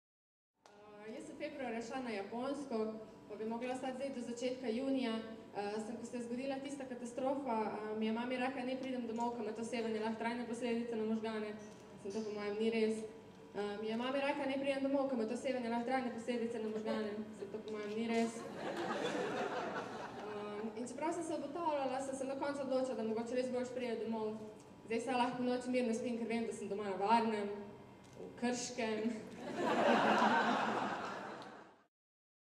StandupLent2011 Iva Lacan.wmv
IVA LAČAN
Iva Lačan se je s stand upom začela ukvarjati leta 2010 po »izobraževanju« na akademiji Panč. V svojih nastopih se na simpatičen način norčuje iz lastne neumnosti, družine, nenavadnih pripetljajev in določenih bizarnih motivov, ki jih najde v medijih, družbi ali jeziku. Piko na i njenim nastopom doda prikupna zmedenost, ki kompenzira njen včasih oster jezik.